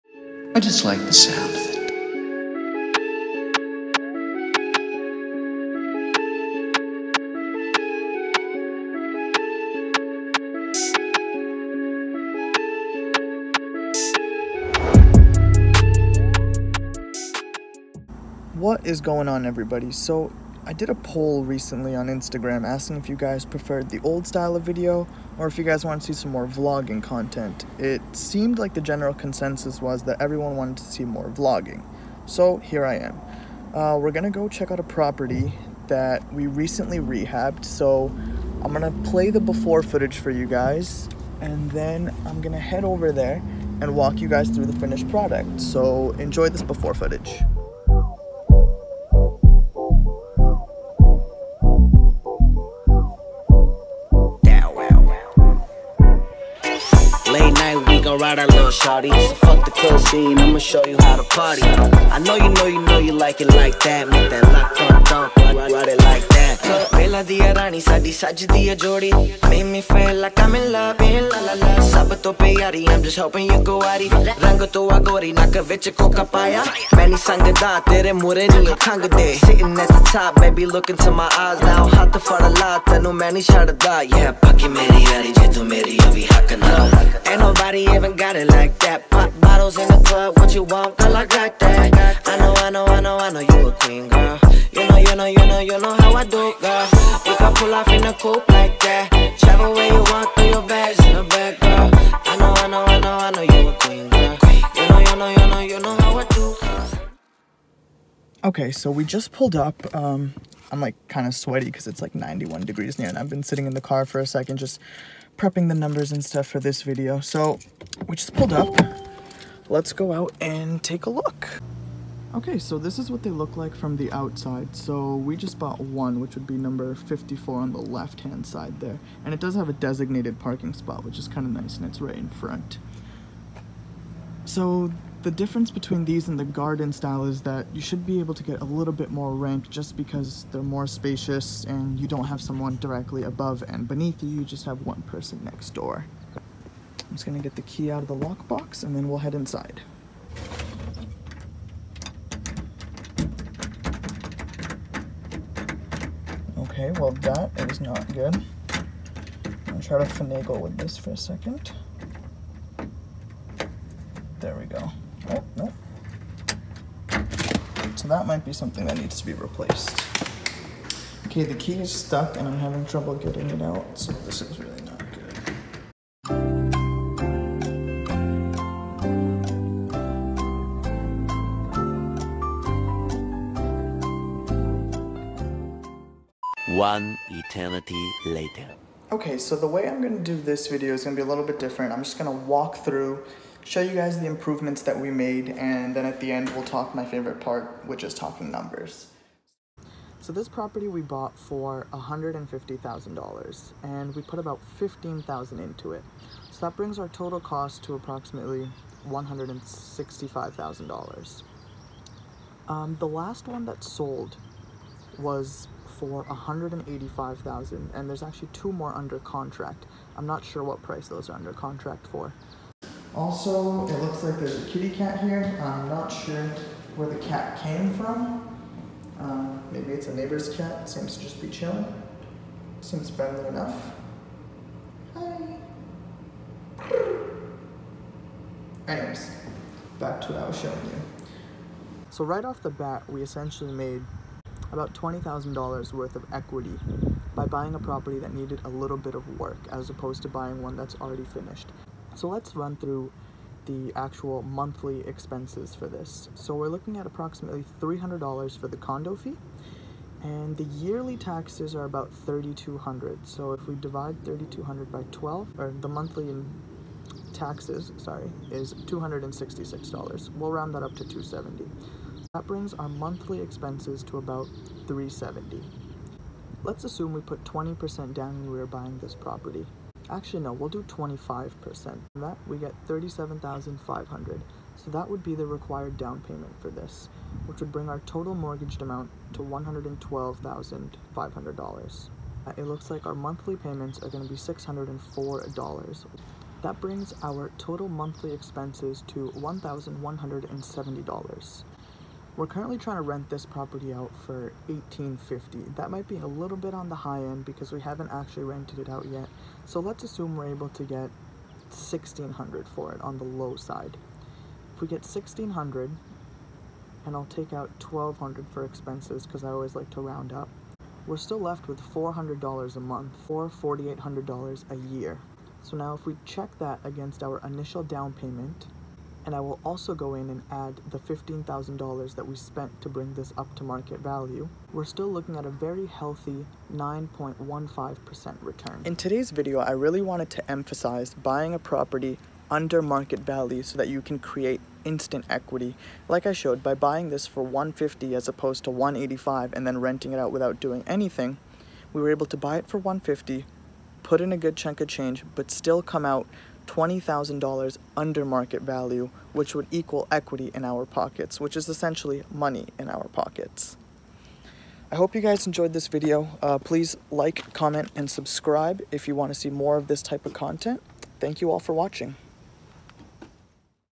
HOW TO MAKE $20,000 IN TWO WEEKS WITH REAL ESTATE
In this video I'll be showing all of you our most recent property rehab. We got this property under market value but that meant it needed some repairs. We replaced the flooring, renovated the bathrooms, and painted the entire unit. By doing so we created $20,000 in equity. We could either sell the property or refinance it to fund the next project.